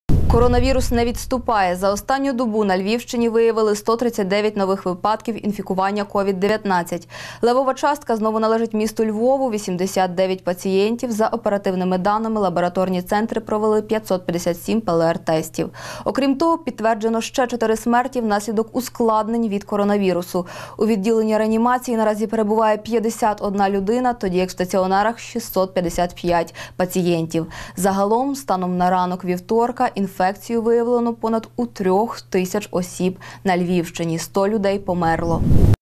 КОРОНАВІРУС НЕ ВІДСТУПАЄ І ВИПУСК НОВИН 16.06.2020
За останню добу на Львівщині виявили 139 нових випадків інфікування COVID-19. Левова частка знову належить місту Львову – 89 пацієнтів.